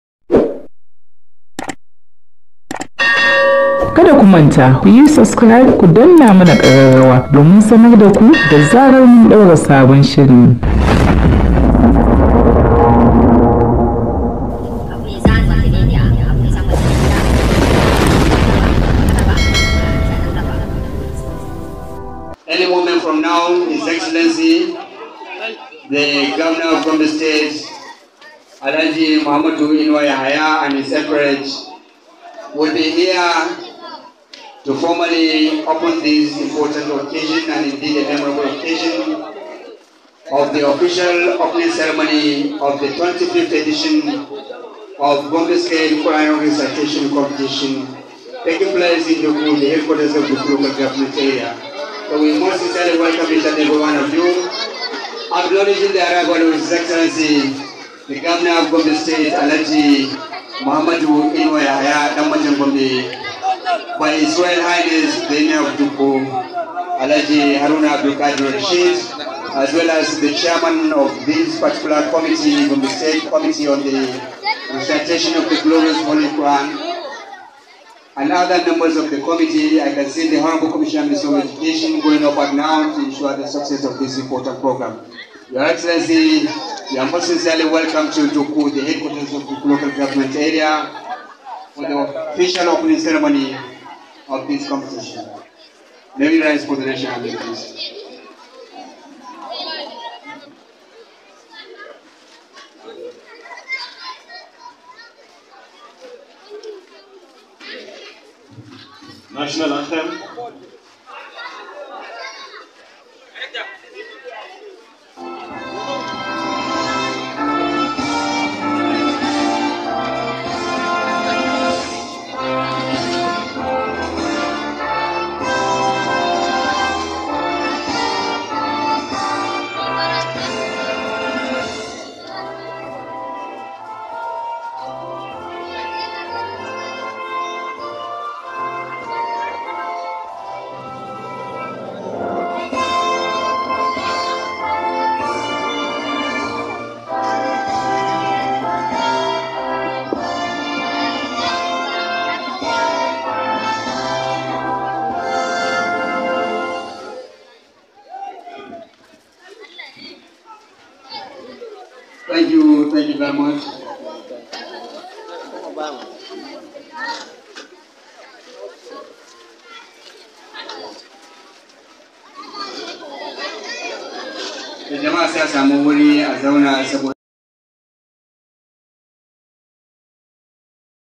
Alh Inuwa Yahya Lokacin isowarsa Dukku don bude Gasar karatu
Gwamnan Gombe a Dukku